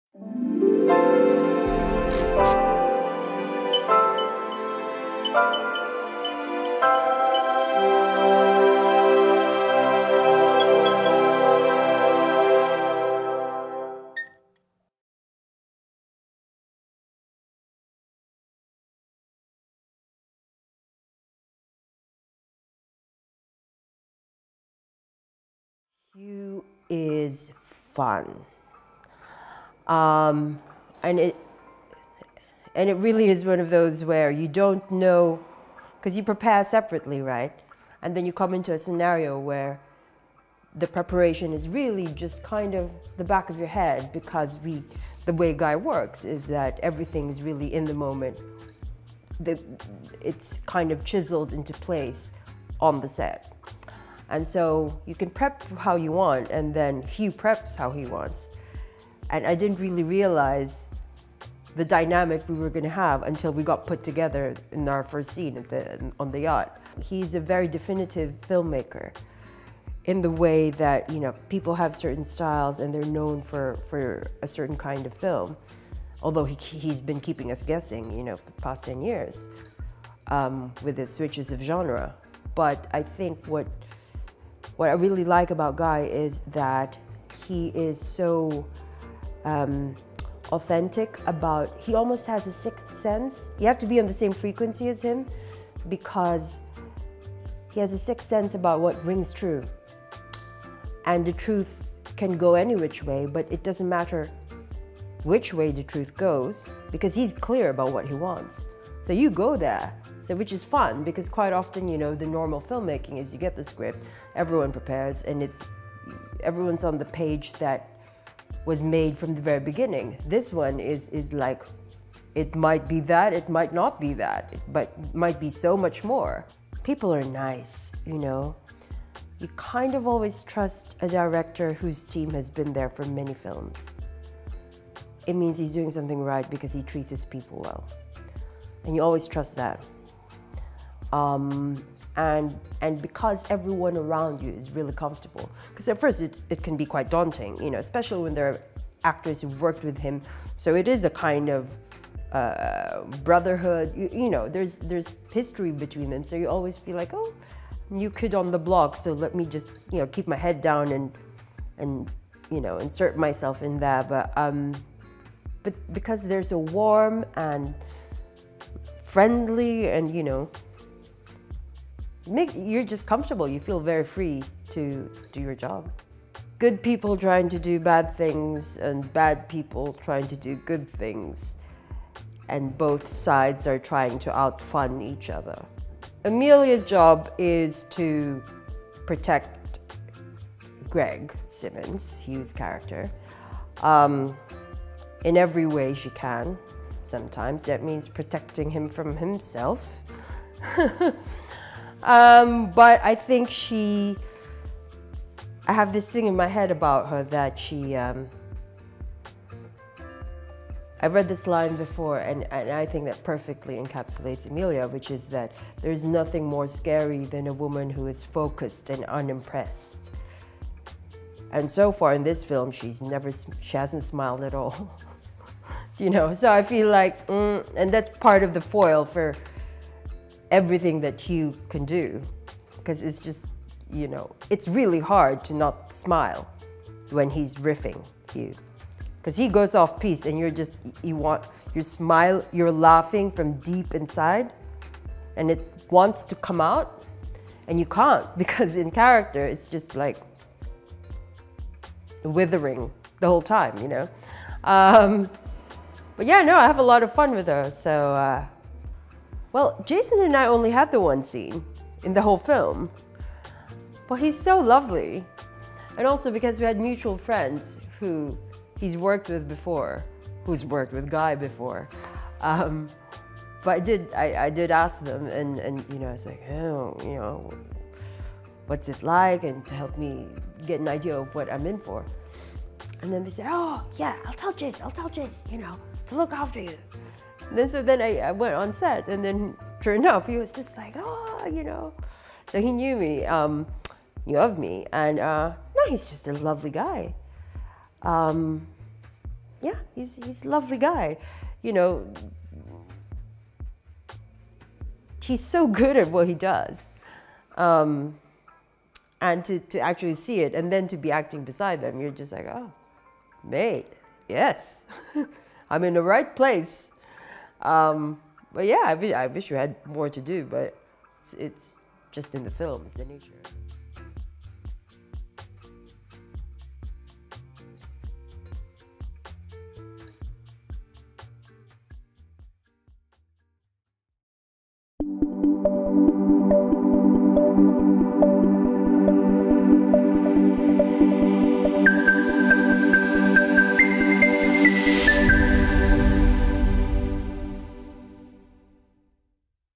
Lourdes Faberes spills secrets on 'Operation Fortune' | In Depth Scoop
🎥Lourdes Faberes speaks on new film 'Operation Fortune'